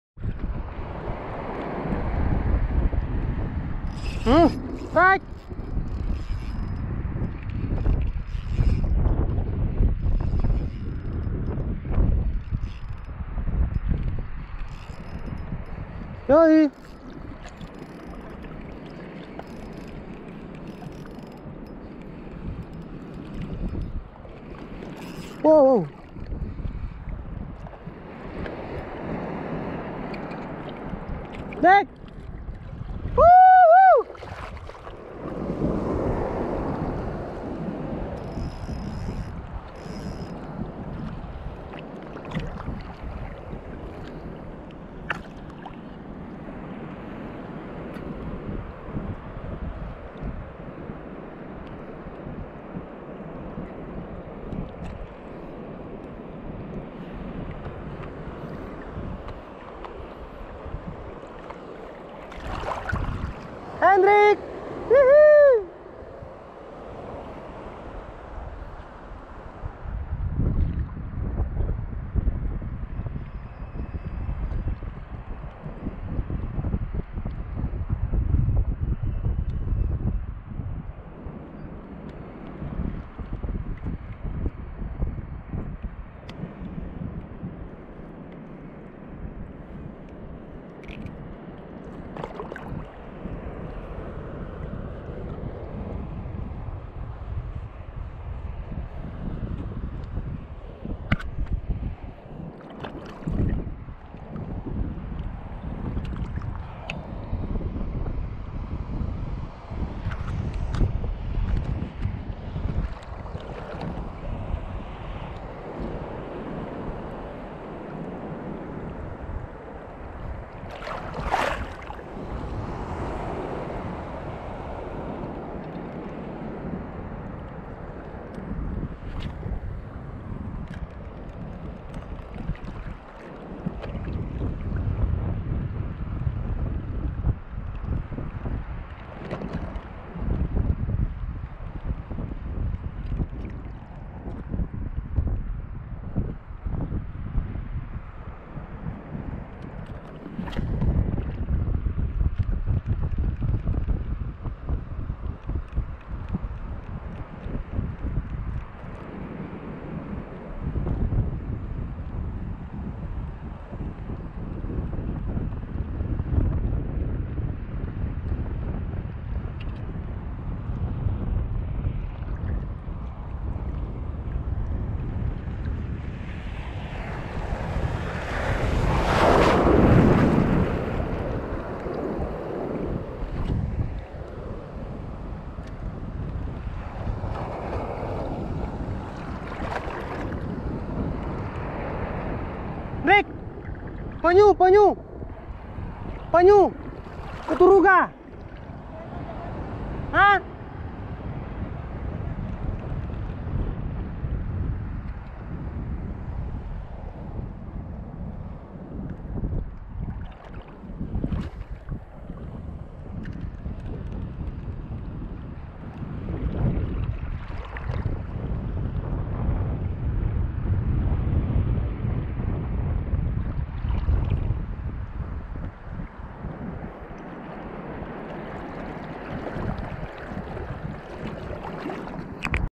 Sensasi Strike dengan Minnow andalan | Ultralight Casting. BIAK TIMUR PAPUA
Set tackle ul.
Rod. Cangkok ul 2-6lbs
Reel. Shimano Sienna 1000fe
Lure. Hinomiya agyle Minnow 6.8grm singking
Pe. Duraking pe 0,8
Leader. 12lbs

Spot biak Papua.
Jangan lupa supportnya guys. Salam satu hobi...